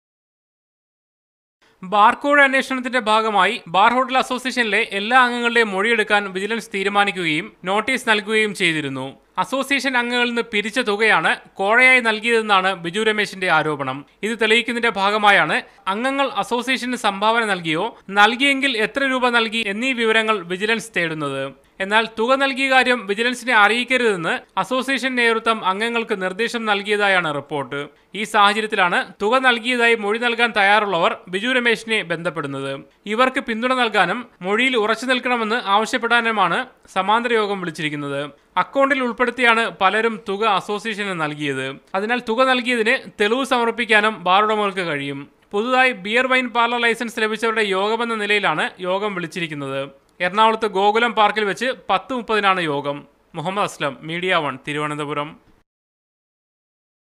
Bar Hotel Association's parallel meeting in Kochi
ബാര്‍ ഹോട്ടല്‍ അസോസിയേഷന്റെ സമാന്തര യോഗം ബിജു രമേശ്​ വിളിച്ചു ചേര്‍ക്കുന്നു. ബിയര്‍, വൈന്‍ ലൈസന്‍സുകാരുടെ യോഗമെന്ന നിലയിലാണ്​ എറണാകുളത്ത് യോഗം വിളിച്ചിരിക്കുന്നത്​. ബാര്‍ കോ‍ഴ സംബന്ധിച്ച് വിജിലന്‍സിന് ​മൊ‍ഴി നല്‍കാന്‍ തയാറുയുള്ളവര്‍ക്ക്‌ പിന്തുണ നല്‍കുകയാണ്​ യോഗത്തിന്റെ ലക്ഷ്യം. അതേസമയം യോഗം ഔദ്യോഗികമല്ലെന്നും പങ്കെടുക്കുന്നവരെ സംഘടനയില്‍ നിന്ന് പുറത്താക്കുമെന്നുമാണ്​ പ്രസിഡന്റ് രാജ്​കുമാര്‍ ഉണ്ണിയുമായി ബന്ധപ്പെട്ടവര്‍ നല്‍കുന്ന സൂചന.